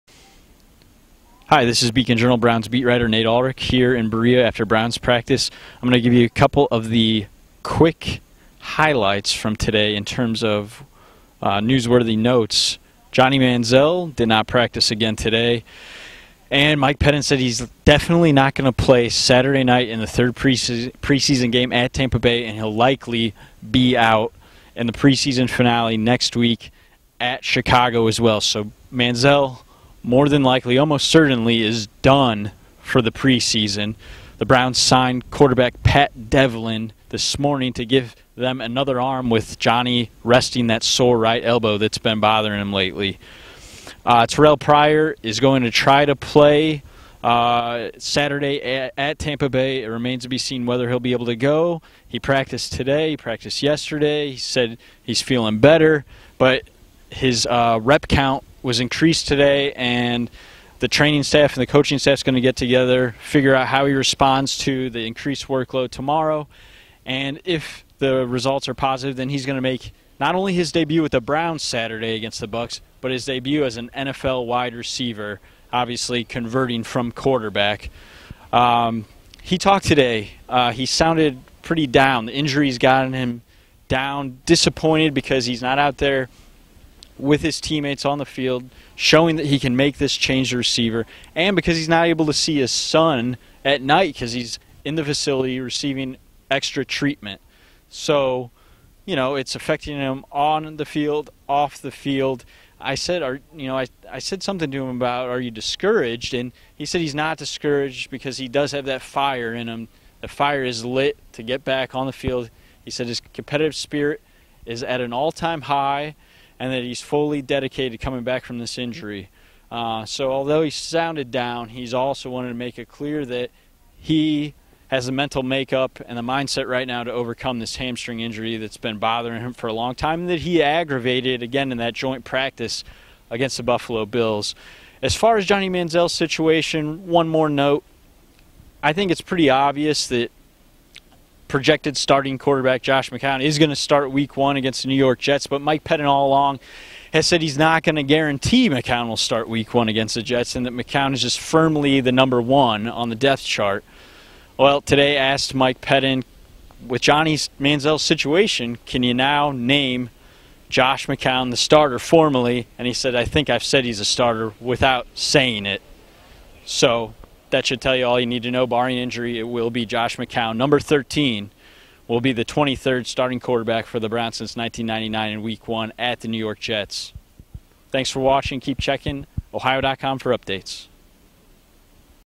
Akron Beacon Journal Browns practice report with Nate Ulrich
Akron Beacon Journal beat reporter Nate Ulrich discusses the decision to rest backup quarterback Johnny Manziel for the rest of the pre-seaon.